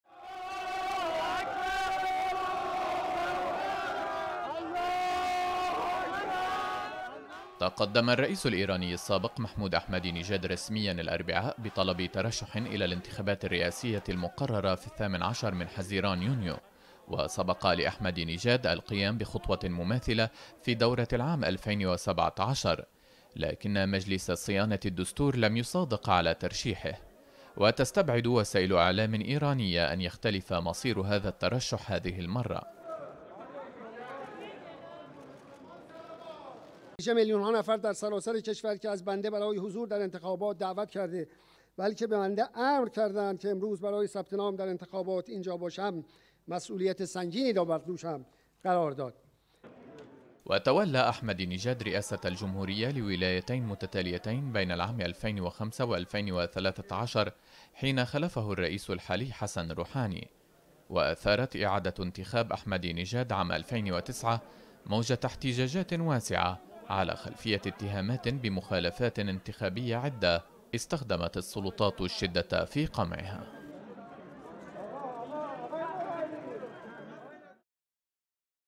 أحمدي نجاد يتقدم بترشيحه مجددا للانتخابات الرئاسية في إيران | AFP
تقدم الرئيس الإيراني السابق محمود أحمدي نجاد رسميا الأربعاء بطلب ترشح الى الانتخابات الرئاسية المقررة في 18 حزيران/يونيو، وفق ما أفاد صحافيون في وكالة فرانس برس.